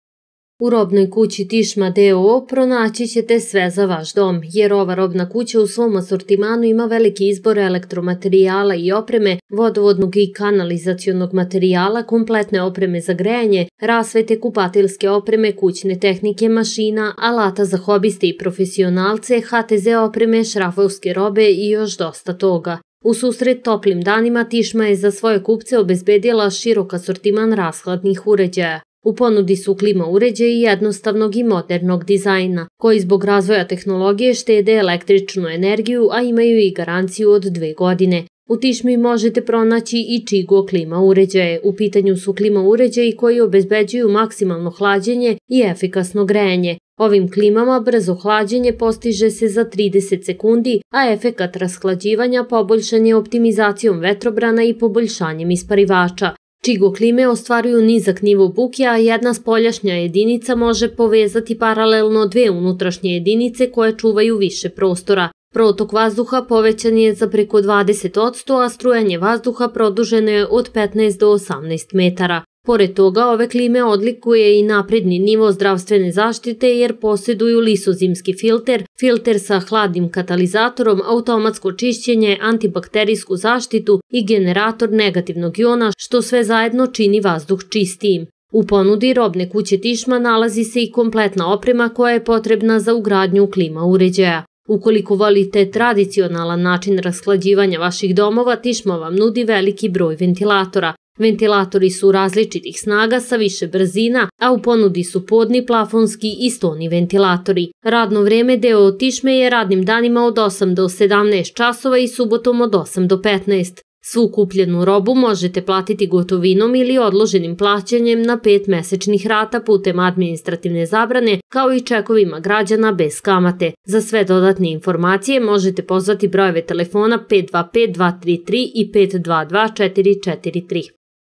Robna kuća "Tišma" nudi rashladne uređaje
Robna kuća "Tišma" iz svog širokog asortimana izdvaja uređaje za rashlađivanje. Podne, plafonske i stone ventilatore, kao i najbolje klime pronaći ćete upravo u robnoj kući "Tišma", koja se nalazi u Beogradskoj ulici bb.